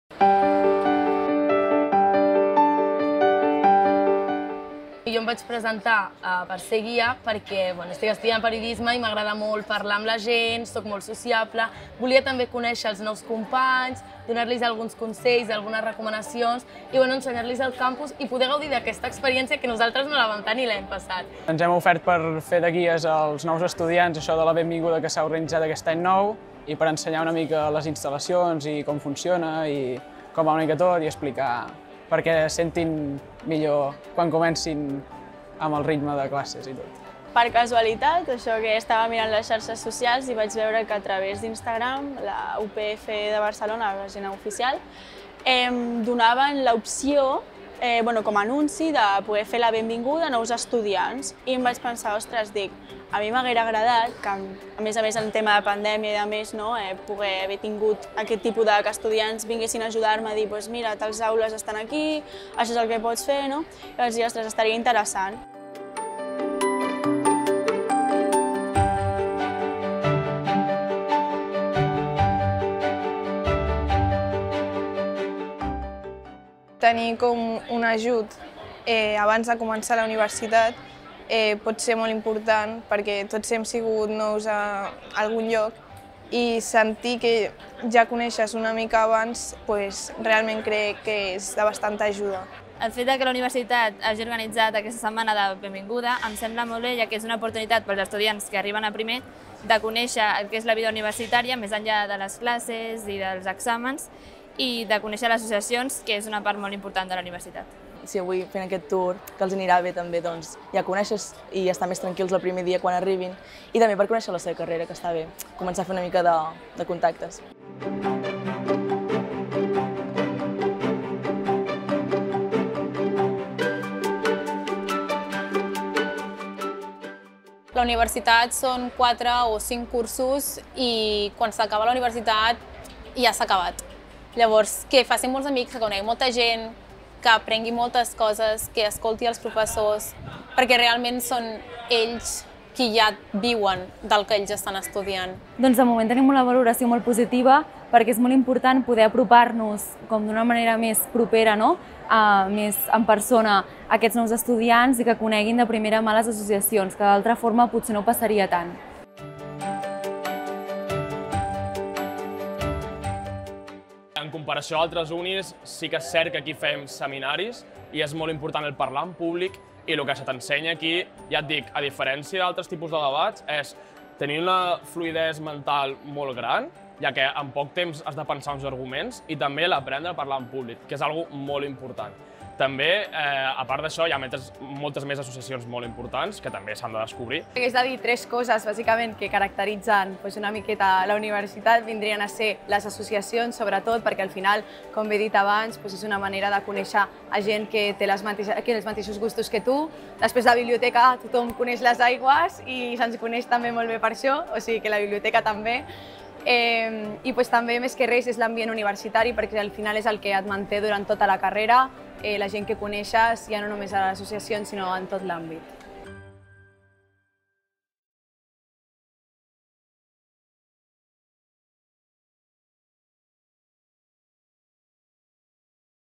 Els guies UPF de La Benvinguda 2022
Prop d'un centenar d'estudiants de grau i d'associacions d'estudiants van participar en La Benvinguda 2022 per als nouvinguts. Durant la setmana del 15 al 23 de setembre van explicar la seva experiència UPF als nous estudiants per ajudar-los a familiaritzar-se amb el ritme Pompeu.